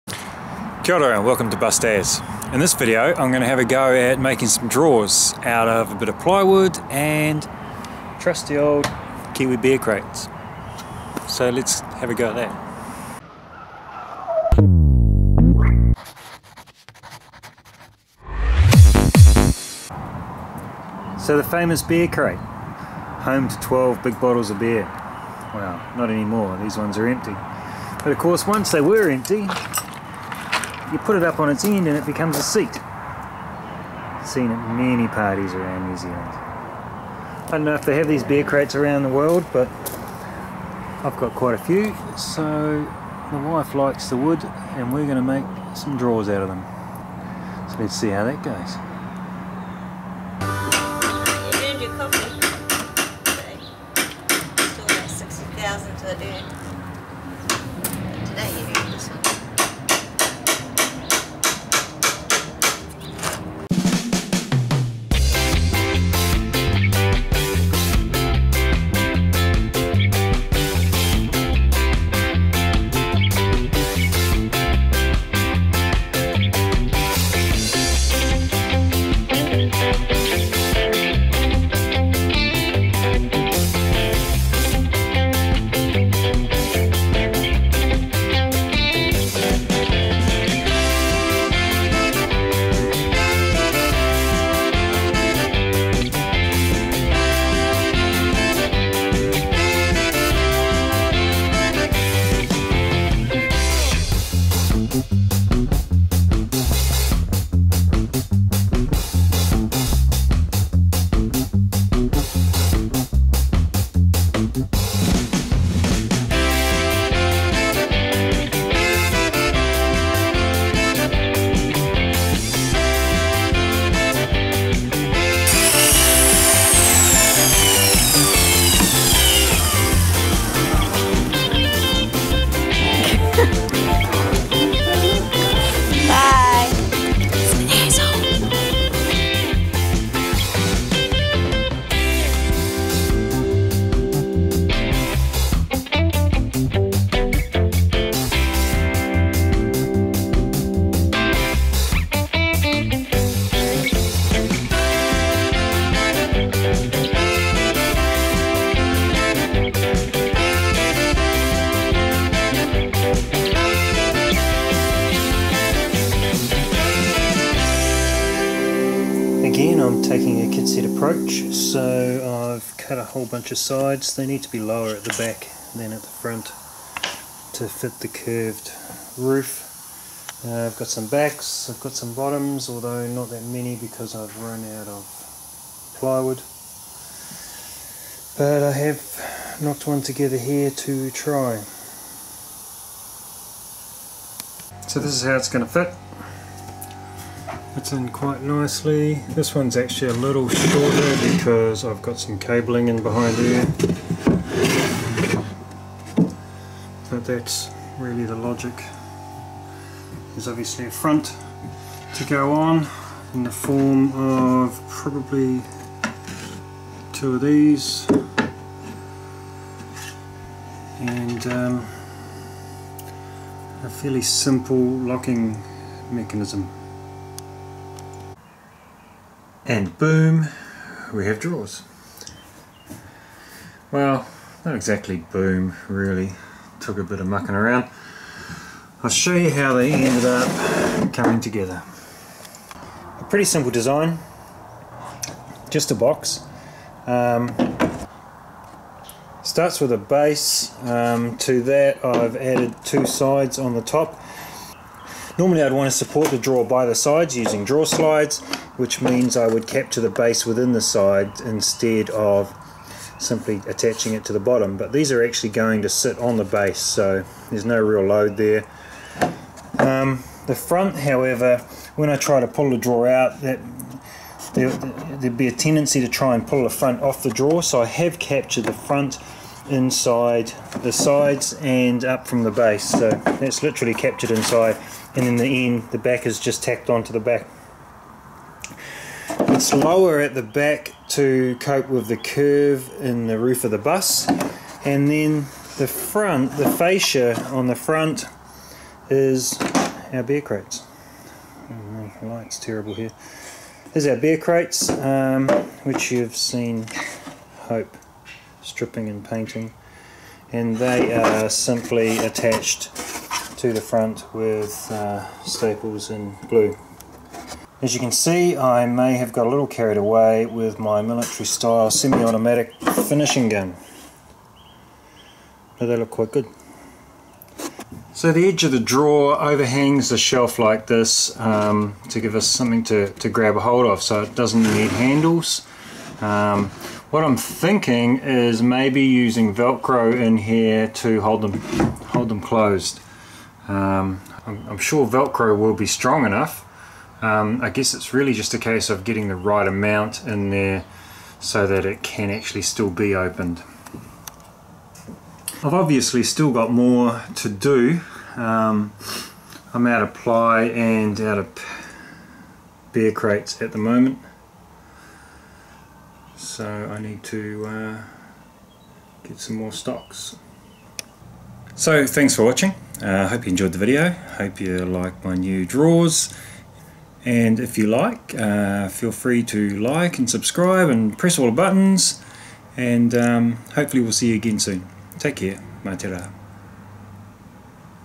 Crate Day Drawers | DIY drawers for the skoolie from beer crates
Crate day, a kiwi tradition, well at least that's what the kids and The Rock radio station tell us. As if we haven't been drinking beer out of crates since well before they were born. Anyway, I have a lot of beer crates and we are going to make them into drawers...
Having built shelves into the top of the bus, I now need some drawers to fill them, plywood of course but to make them look cool I'm going with beer crates. Lion Brown, Lion red, Speights they beer is different but the crates are all the same, even my favourite Steinlager comes in big bottles in these handy crates. Handy for storage, sitting on, even keeping warm on the campfire, and now for drawers.

We are Bused_As
We will show you the good bits and not so good bits, once a week, right here in Auckland, New Zealand